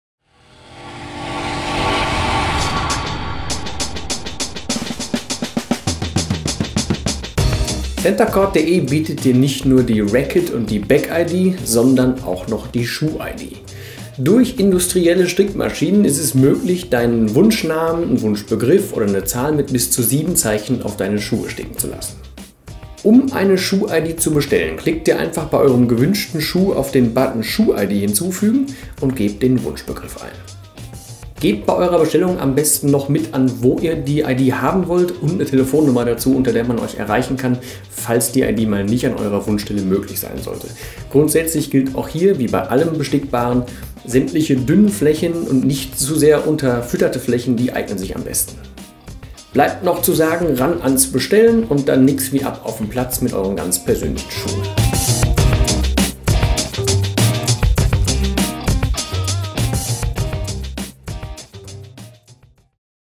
SHOE iD - Personalisiere deine Tennisschuhe
Als einer der größten Tennisshops Europas bietet dir CenterCourt.de neben einer riesigen Auswahl an Tennisschuhen jetzt auch die SHOE iD an. Damit kannst du mit nur wenigen Mausklicks deine Tennisschuhe professionell besticken lassen. Es war noch nie so einfach, ein echtes Unikat zu erschaffen! Die SHOE iD kannst du direkt im Onlineshop von CenterCourt.de hinzufügen. Suche dir dazu einfach deine Tennisschuhe aus und klicke danach auf „SHOE iD hinzufügen". Nun stehen dir bis zu 7 Zeichen (je nach Schuhtyp) zur Verfügung, um deinen Tennisschuh zu personalisieren. Deiner Kreativität sind dabei keine Grenzen gesetzt. Ob Namen der Tennisprofis, des Tennisvereins oder dein eigener Name oder Spitzname: die SHOE iD bietet dir jede denkbare Möglichkeit der Individualisierung. Grundsätzlich aber gilt: umso weniger Zeichen, desto besser. Bei einigen Tennisschuhen ist eine Bestickung leider nicht möglich. In diesem Fall wird der Button für die SHOE iD nicht angezeigt. Unser Experten-Team sucht den perfekten Platz auf den Tennisschuhen aus. Mittels industriellen Profi-Maschinen wird deine persönliche SHOE iD dann sorgfältig auf die Tennisschuhe gestickt. Noch ein paar Handgriffe für das Finish und fertig sind deine ganz persönlichen Tennisschuhe.

Unser Tipp: gerade zu Weihnachten oder zum Geburtstag ist die Personalisierung mit der SHOE iD ein wirklich tolles und einzigartiges Geschenk. Probiert es einfach einmal selbst aus.